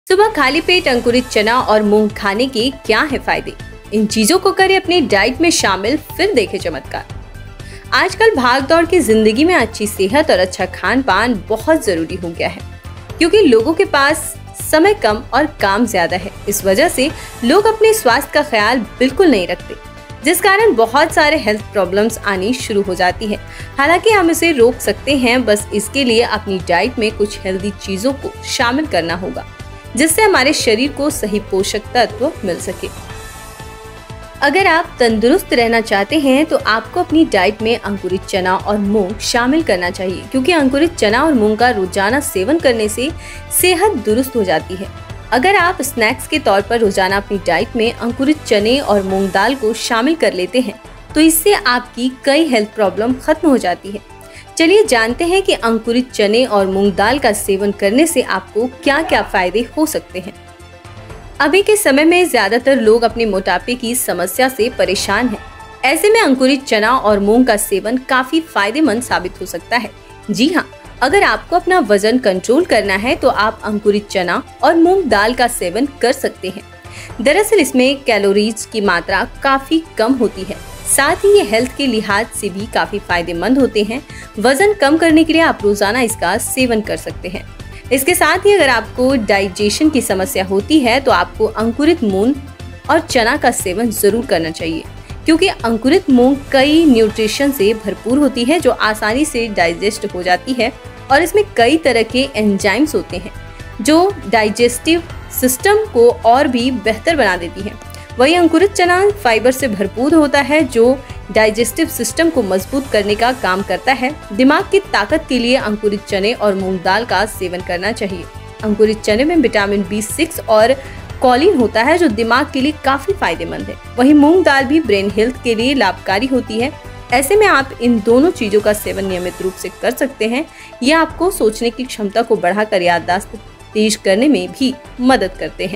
Good Health: सुबह खाली पेट अंकुरित चना और मूंग खाने के क्या है फायदे?
Boost Your Health with Sprouted Gram and Moong: Benefits of Eating Them on an Empty Stomach!

Including sprouted gram and moong in your daily diet, especially on an empty stomach in the morning, can offer numerous health benefits. From weight management and improved digestion to enhanced brain power and boosted immunity, these sprouts can be a simple yet powerful addition to your healthy lifestyle.